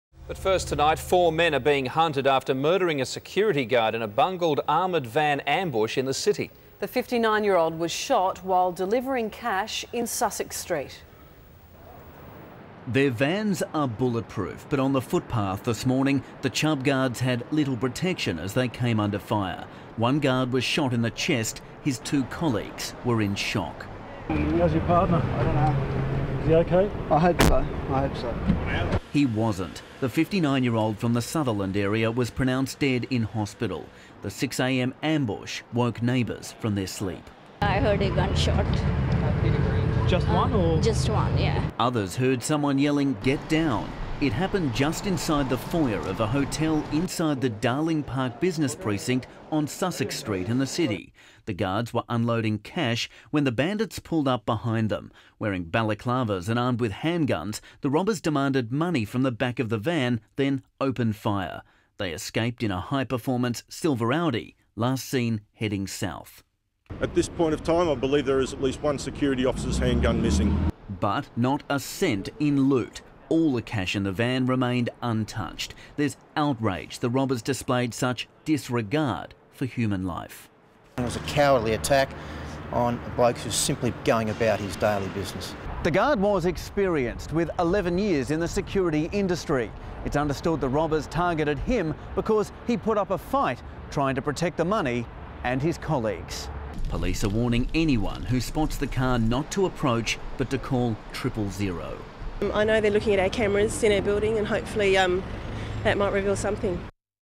Deadly Ambush
A security guard shot during a bungled robbery in Sydney's CBD this morning